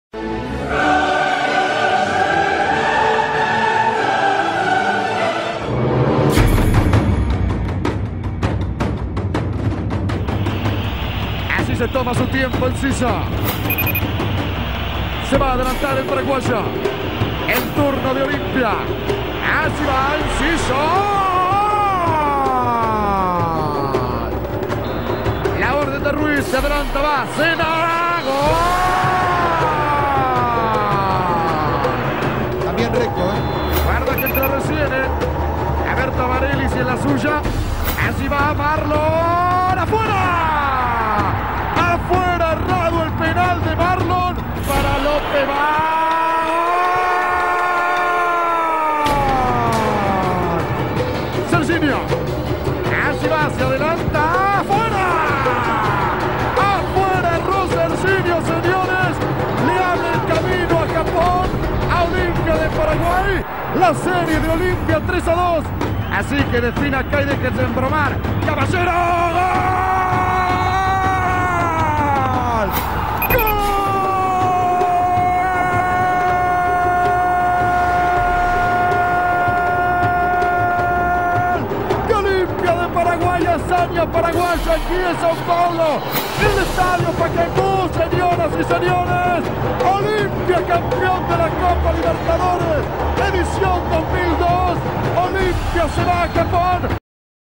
Olimpia vs Sao Caetano Final Copa Libertadores 2002 (penales)
Definición por penales de la final de la Copa Libertadores de América del Año 2002 ganada por OLIMPIA!